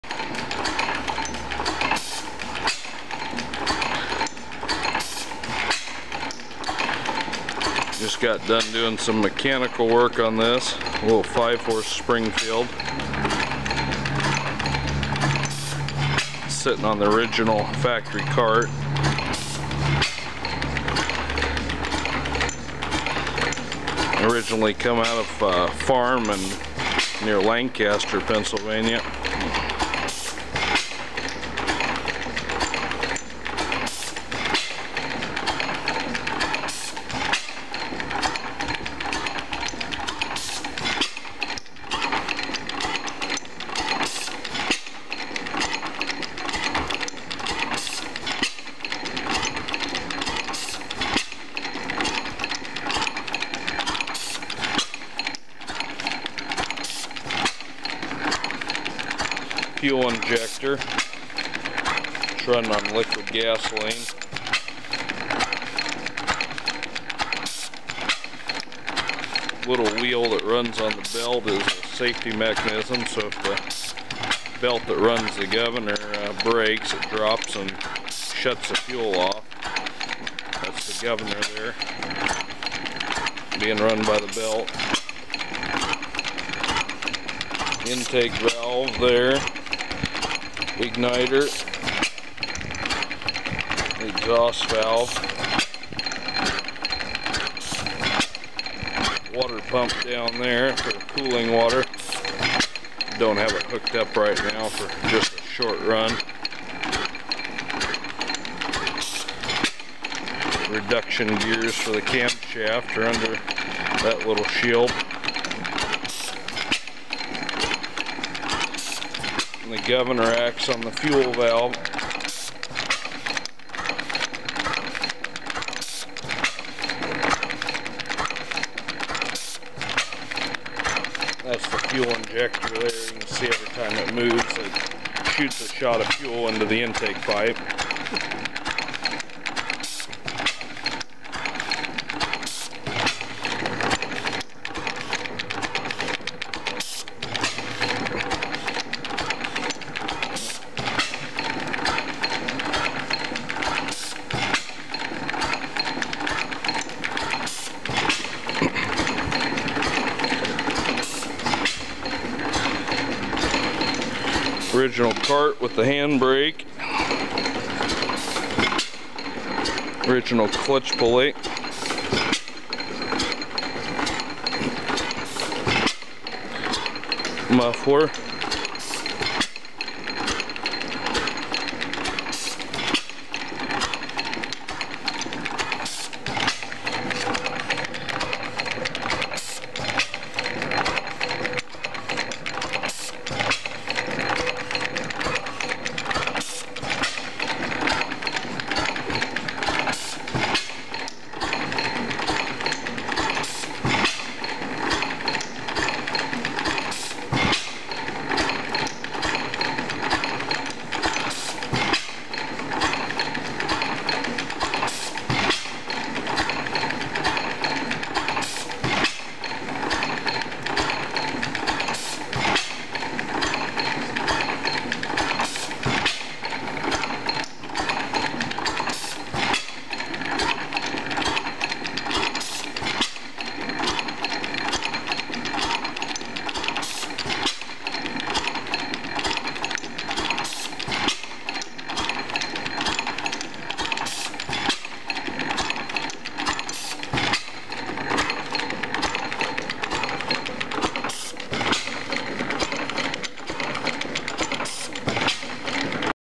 5 HP Springfield Gas Engine
5 HP Springfield gas engine built in Springfield, Ohio. The engine came from a farm in Lancaster Co. PA. It's on the original transport, with brake, clutch, water pump, and muffler. Unique features include dual sideshaft, overhead cam, fuel injection controlled by hit and miss scavenge governing system, leaf spring igniter trip, dual spring exhaust valve, and shaft driven water pump.